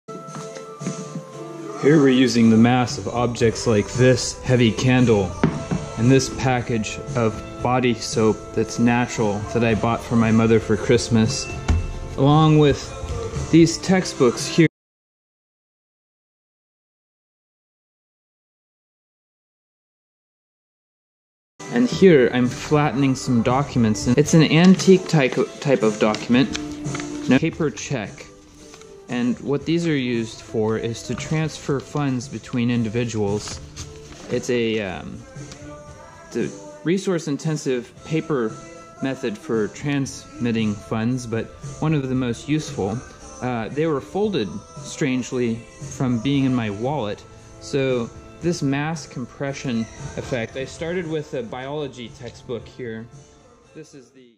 Check Flatten With Heavy Books & Objects
Using heavy books & a candle, I flatten some personal checks that were bent from being stored in my trifold wallet.